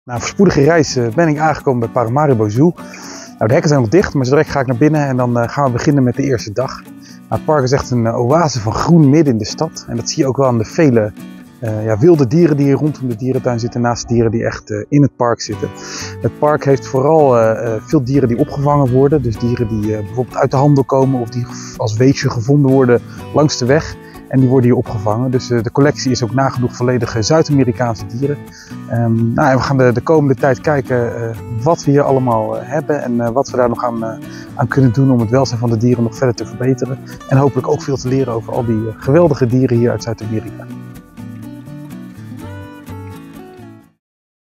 Steven naar Paramaribo Zoo | Vlog 2
ZooParc's hoofddierenverzorger Steven van den Heuvel is momenteel een aantal weken te gast in Paramaribo Zoo. Hij hoopt hier veel te leren over de dieren in Zuid-Amerika: de verzorging, de natuurlijke leefomgeving en de uitdagingen die er spelen. Ook zal hij de dierentuin ondersteunen en adviseren. Steven neemt ons mee op zijn reis door middel van vlogs. In deze tweede aflevering is hij net aangekomen en deelt hij zijn eerste indruk met ons.